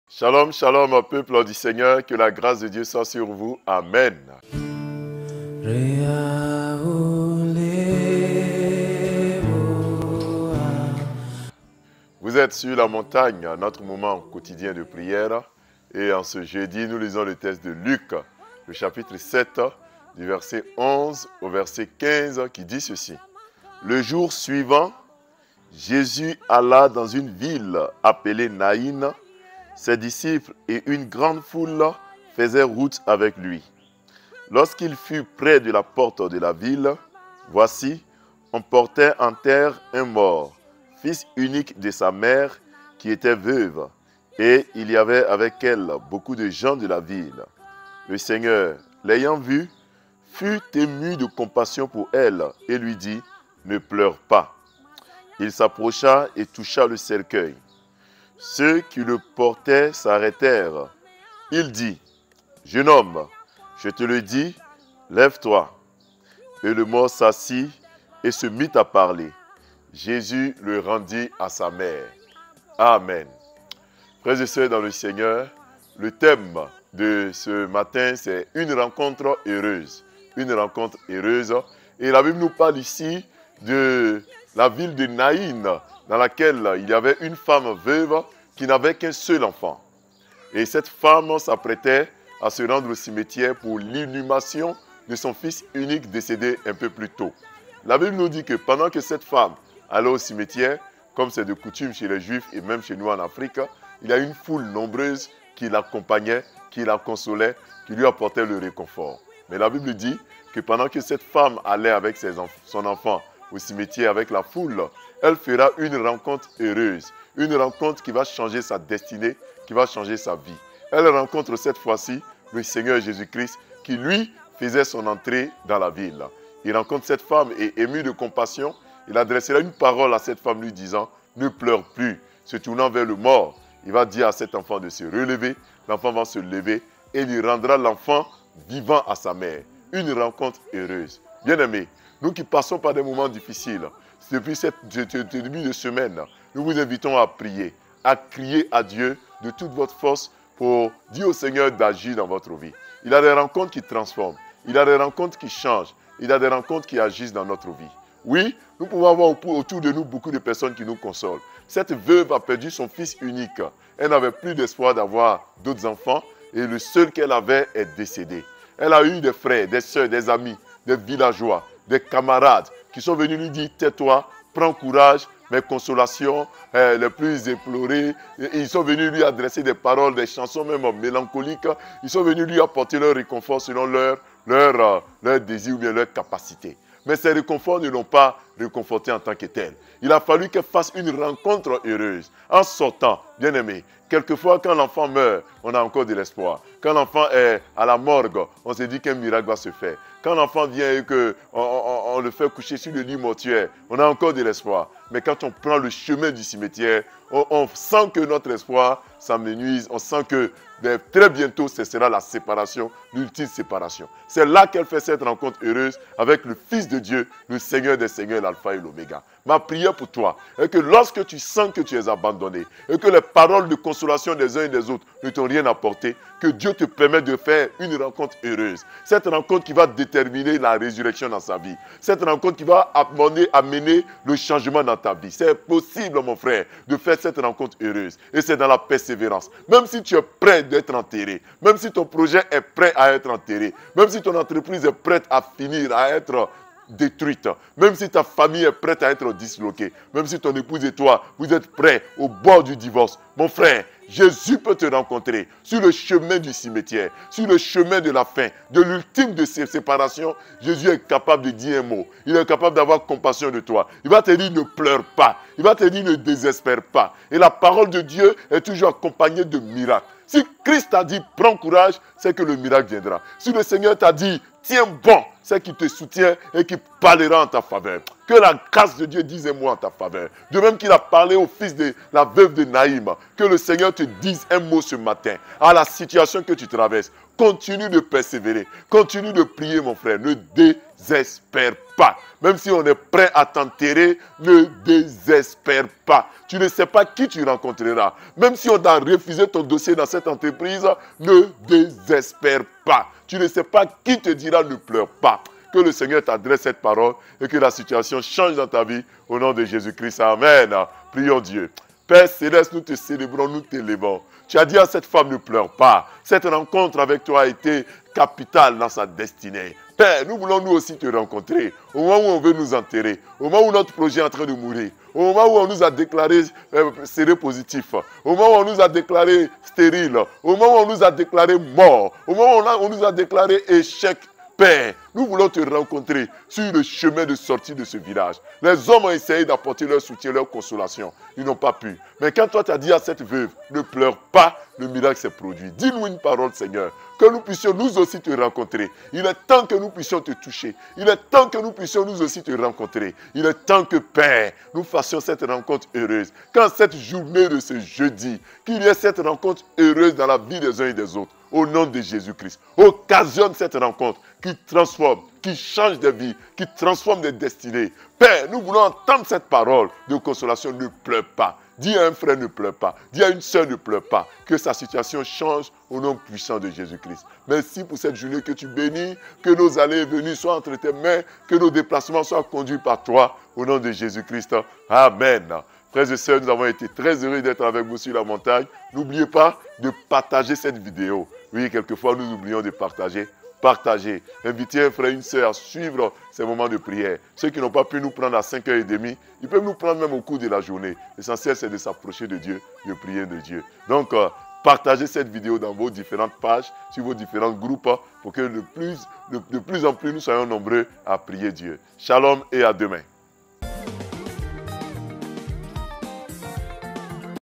𝗦𝗨𝗥 𝗟𝗔 𝗠𝗢𝗡𝗧𝗔𝗚𝗡𝗘Thème : Une rencontre heureuse. Luc 7:11-15 Pasteur Ehua De Monga
𝗦𝗨𝗥 𝗟𝗔 𝗠𝗢𝗡𝗧𝗔𝗚𝗡𝗘
Thème : Une rencontre heureuse. Luc 7:11-15
Pasteur Ehua De Monga

Prière pour confier sa journée à Dieu.